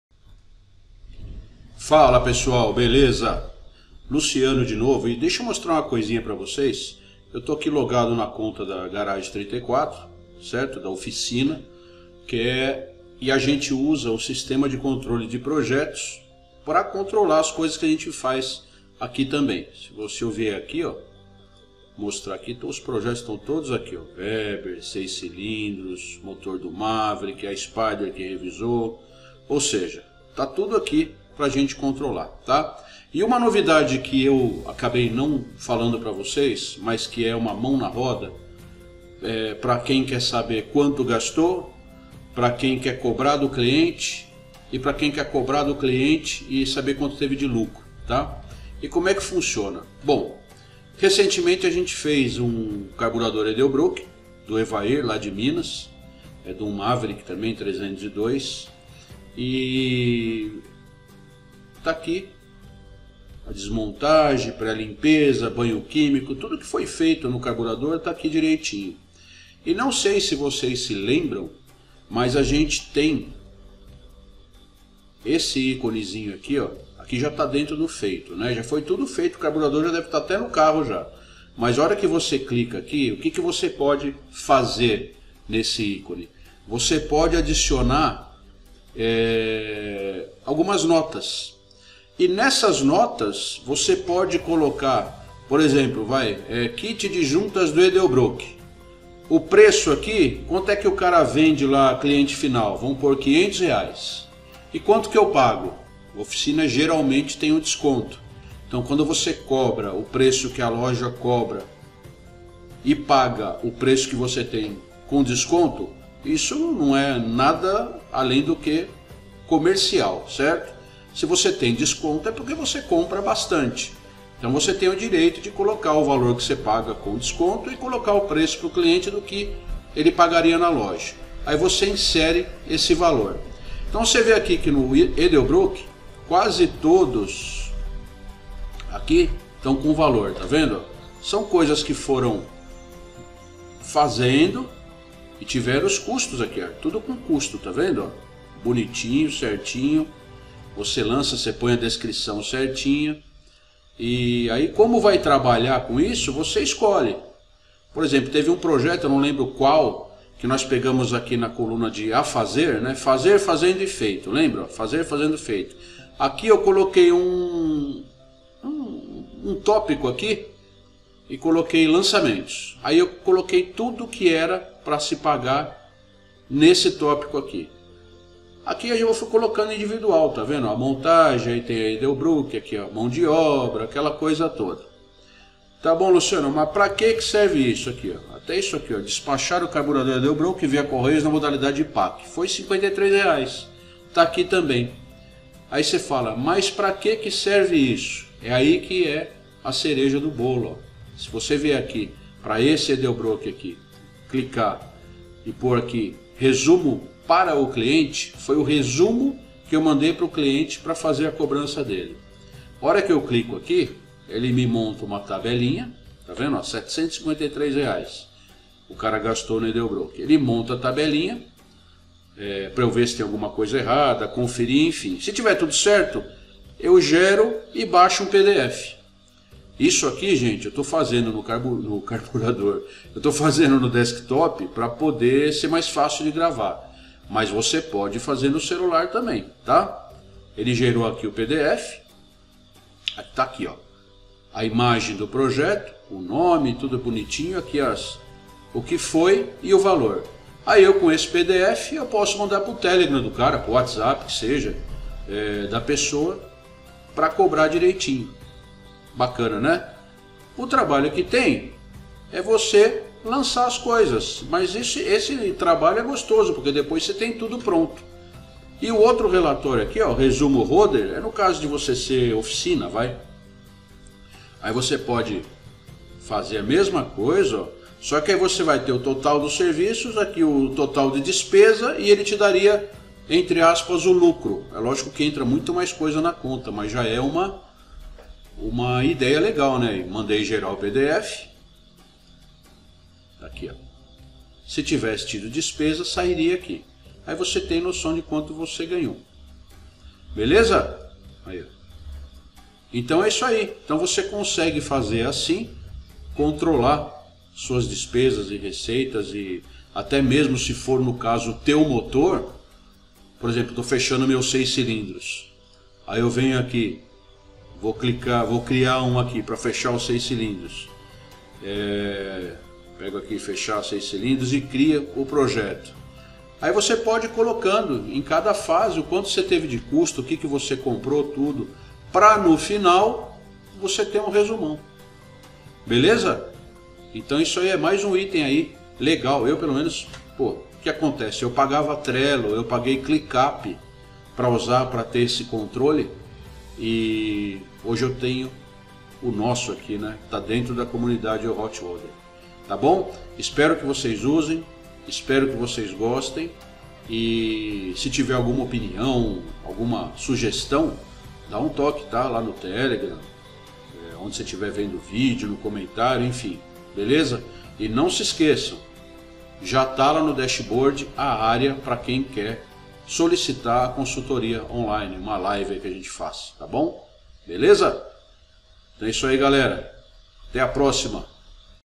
NA COMUNIDADE EUHOTRODDER VOCÊ PODE INSERIR VALORES NAS FASES DO PROJETO E, NO FINAL, EMITIR UM PDF
O fato de você ter um sistema estilo KANBAN dentro do nosso App já é bem legal.

Agora, imagina você tendo como colocar valores nas fases e, ao final, emitir um PDF para você ou seu cliente?

Simplesmente o máximo!

Eu que paguei o trello por anos, agora utilizo o meu próprio sistema de controle de Projetos online...

Liberei o TEST DRIVE dentro da comunidade EuHotRodder.com

Acesse e veja todas as possibilidades dentro do nosso App.

Valeu!